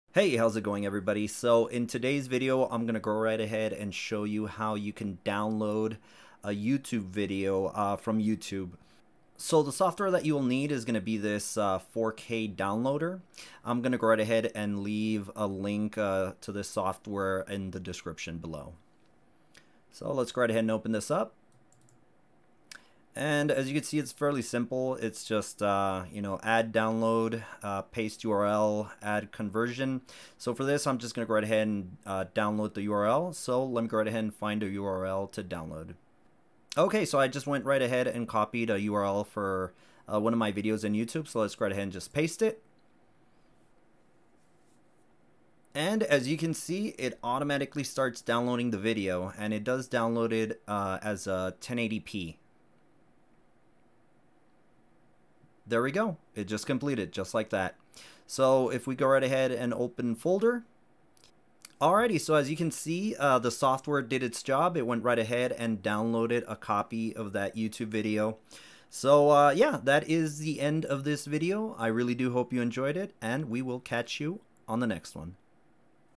MP4 YouTube video download
How to download a YouTube video and save it as a .mp4.

Tools & Gear I Use:
Editing Software